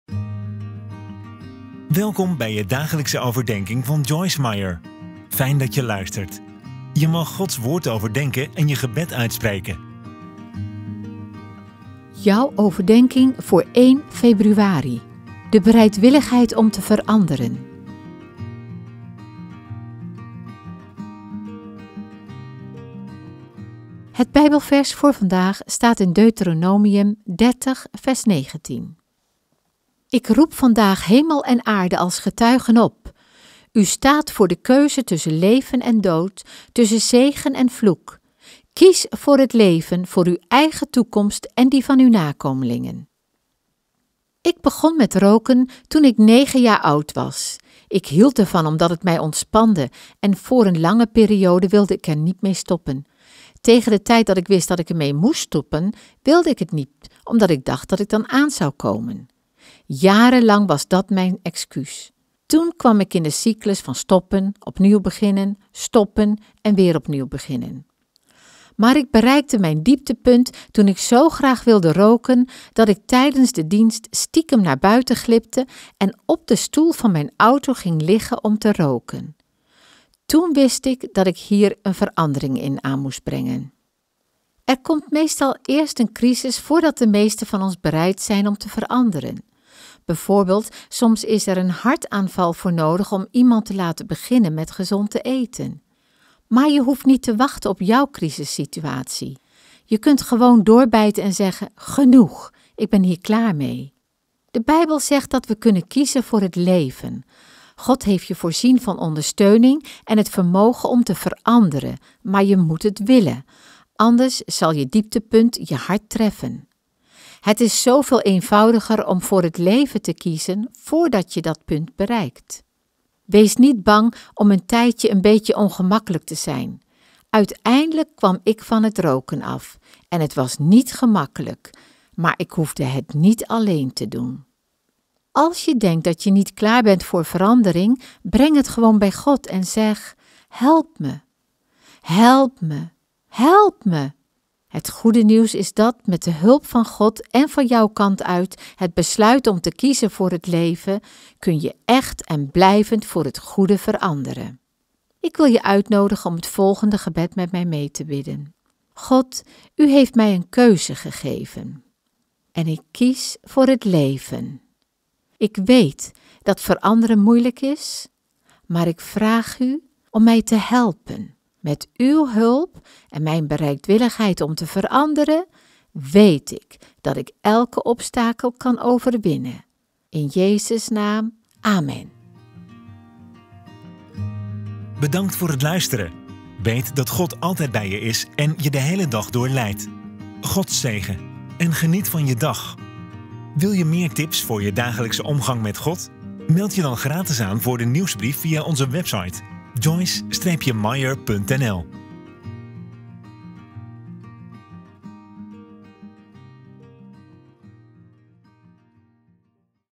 De bereidwilligheid om te veranderen | Overdenking | Joyce Meyer
Ik begon met roken toen ik negen jaar oud was. Ik hield ervan omdat het mij ontspande en, voor een lange periode, wilde ik er niet mee stoppen. Tegen de tijd dat ik wist dat ik ermee moest stoppen, wilde ik het niet omdat ik dacht dat ik dan zou aankomen.

00:00 Dagelijkse overdenking van Joyce Meyer van 1 februari
00:15 De bereidwilligheid om te veranderen
00:25 Deuteronomium 30;19 (NBV)
Ik roep vandaag hemel en aarde als getuigen op: u staat voor de keuze tussen leven en dood, tussen zegen en vloek. Kies voor het leven, voor uw eigen toekomst en die van uw nakomelingen.
00:40 Overdenking
Ik begon met roken toen ik negen jaar oud was. Ik hield ervan omdat het mij ontspande en, voor een lange periode, wilde ik er niet mee stoppen. Tegen de tijd dat ik wist dat ik ermee moest stoppen, wilde ik het niet omdat ik dacht dat ik dan zou aankomen. Jarenlang was dat mijn excuus. Toen kwam ik in de cyclus van stoppen, opnieuw beginnen, stoppen en weer opnieuw beginnen.

Maar ik bereikte mijn dieptepunt toen ik zo graag wilde roken dat ik tijdens de dienst stiekem naar buiten glipte en op de stoel van mijn auto ging liggen om te roken. Toen wist ik dat ik hier een verandering in aan moest brengen.

Er komt meestal eerst een crisis voordat de meesten van ons bereid zijn om te veranderen. Bijvoorbeeld, soms is er een hartaanval voor nodig om iemand te laten beginnen met gezond te eten. Maar je hoeft niet te wachten op jouw crisissituatie. Je kunt gewoon doorbijten en zeggen: “Genoeg. Ik ben hier klaar mee!”

De Bijbel zegt dat we kunnen kiezen voor het leven. God heeft je voorzien van ondersteuning en het vermogen om te veranderen, maar je moet het willen, anders zal je dieptepunt je hard treffen. Het is zoveel eenvoudiger om voor het leven te kiezen voordat je dat punt bereikt.

Wees niet bang om een tijdje een beetje ongemakkelijk te zijn. Uiteindelijk kwam ik van het roken af, en het was niet gemakkelijk… maar ik hoefde het niet alleen te doen.

Als je denkt dat je niet klaar bent voor verandering, breng het gewoon bij God en zeg: “Help me, help me, help me.”

Het goede nieuws is dat, met de hulp van God en van jouw kant uit het besluit om te kiezen voor het leven, kun je echt en blijvend voor het goede veranderen.

02:00 Kort gebed
God, U heeft mij een keuze gegeven, en ik kies voor het leven! Ik weet dat veranderen moeilijk is, maar ik vraag U om mij te helpen. Met Uw hulp en mijn bereidwilligheid om te veranderen, weet ik dat ik elke obstakel kan overwinnen.